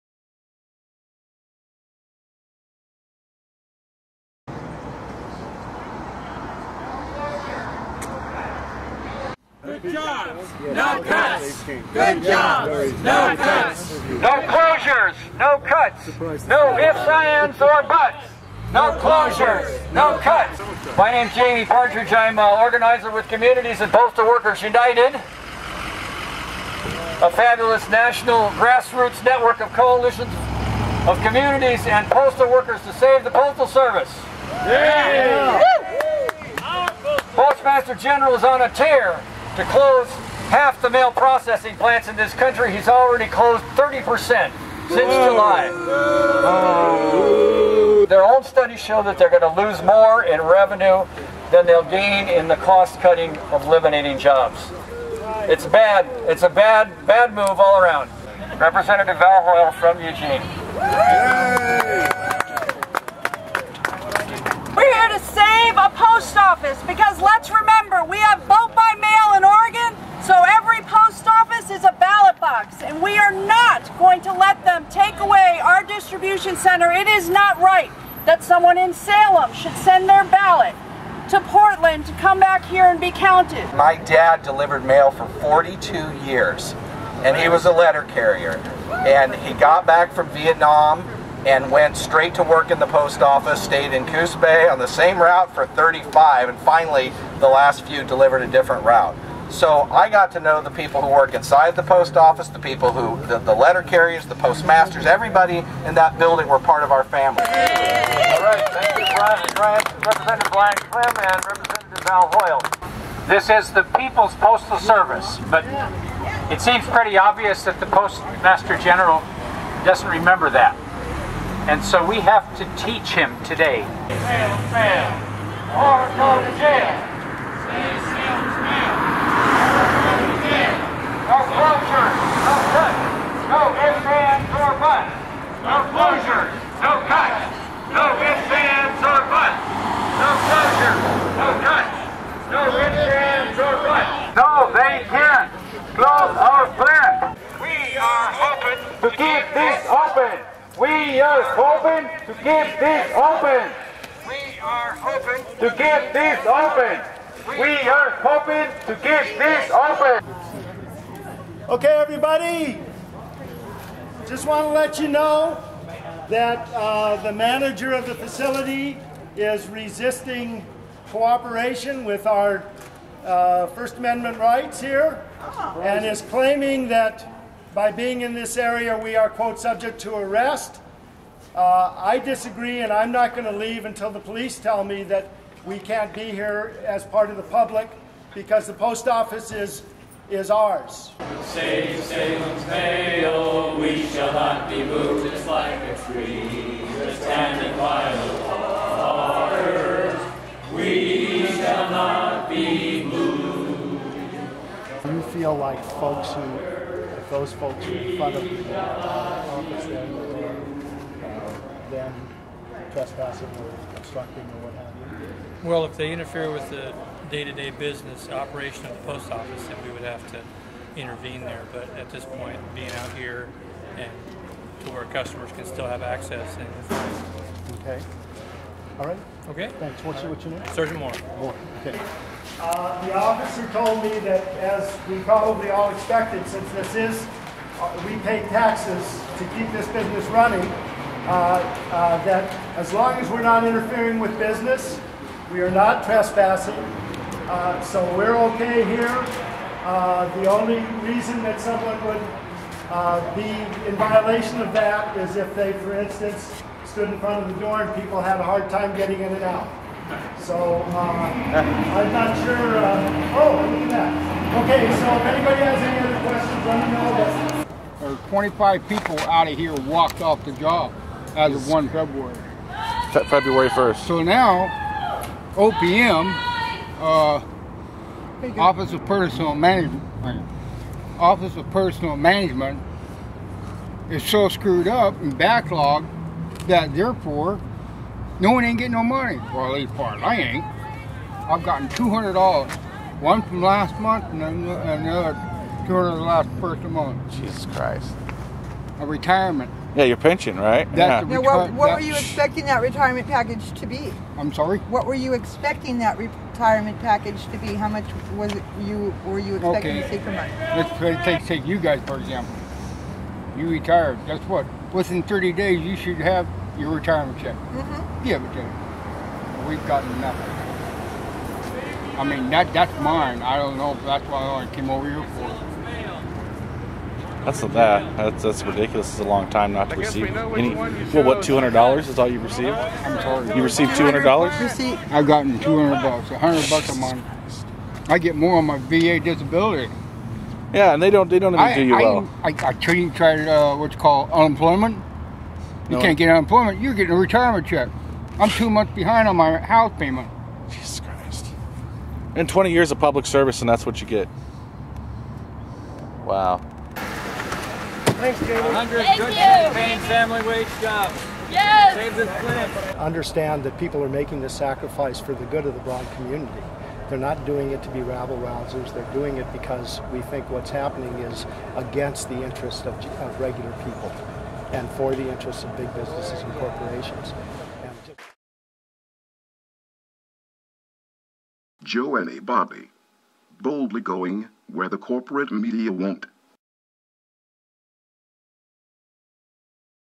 5 Arrested Protesting Closure Of Salem Mail Processing Facility (Short) 4.17.13
An oregon "Save Americas Postal Service" campaign action led
by Jamie Partridge of Portland Communities and Postal Workers
United and Greg Margolis of Portland Jobs With Justice. Around
seventy protesters held a rally in front of the Salem, OR USPS
mail processing plant and were joined by area state legislative
representatives which spoke of their concern for saving this
public service. They point out that they are pushing a bill to
congress (The Postal Service Protection Act Of 2013) to save
this vital american institution, and the jobs it has provided for
so many. Protesters wrapped up the rally and announced that
they would be marching to the back of the mail plant to a
public drop-off entrance to occupy the premises and request
an answer from someone in charge of closing the facility.

  After a stand off with postal inspector police, salem police were
called and responded. They spoke with a liason (Greg Margolis)
and stated that everyone was free to express their first
amendment rights as long as they did not interrupt service.
Five designated individuals including Jamie Partridge and John
Schweibert decided to enter the building and confront operations
management about the scheduled closure in June. These
protesters were arrested and spent close to ten hours in book,
hold, and release in the marion county jail. They recieved
complaints of tresspassing charged by the Post Office and will
have court dates soon. This is part of a national campaign
known as "Save Americas Postal Service".